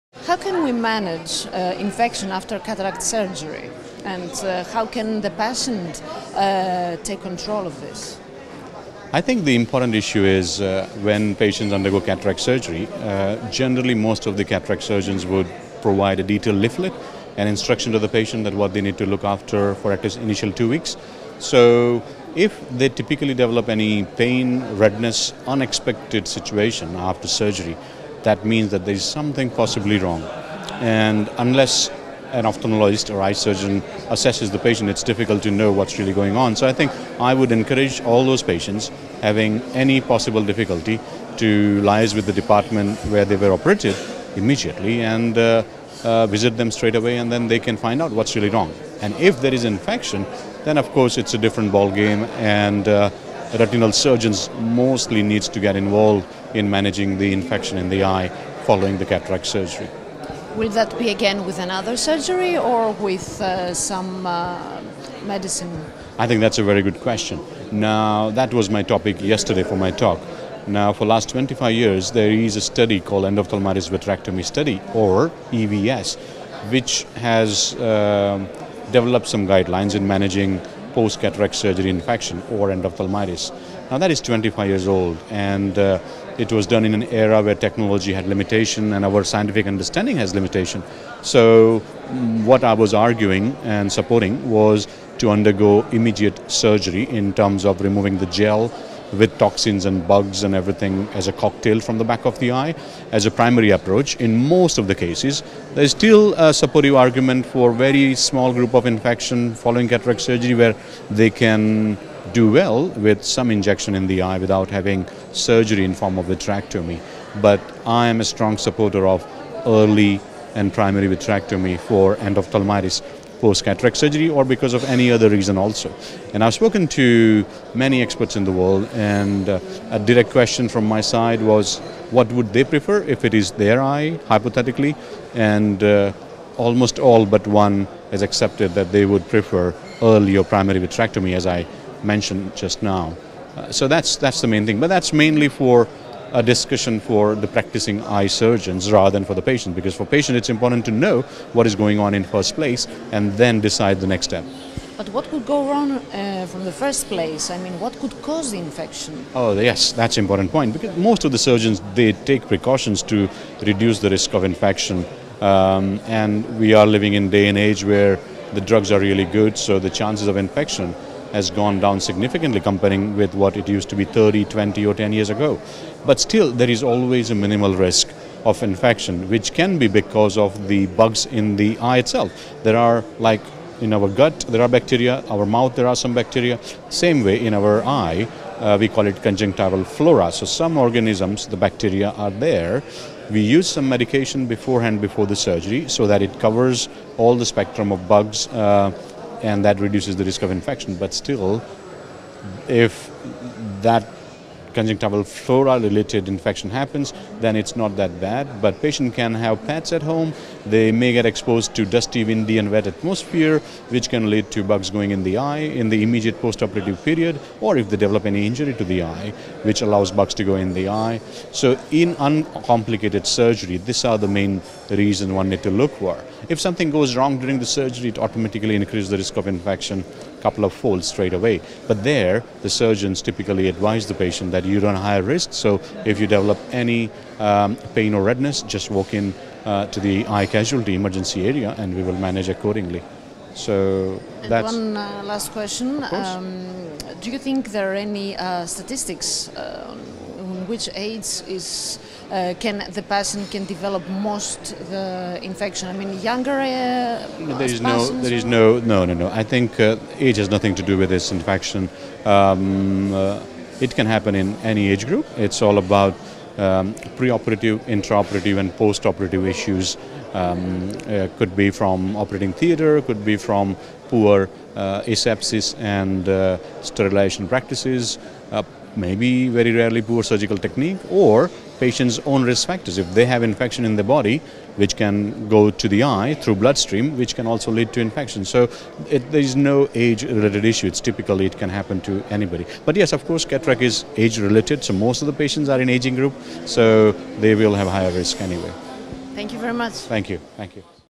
Malhar Soni | Retina Surgeon
Malhar Soni - How can we manage infection after a cataract surgery |  51st Panhellenic Ophthalmological Conference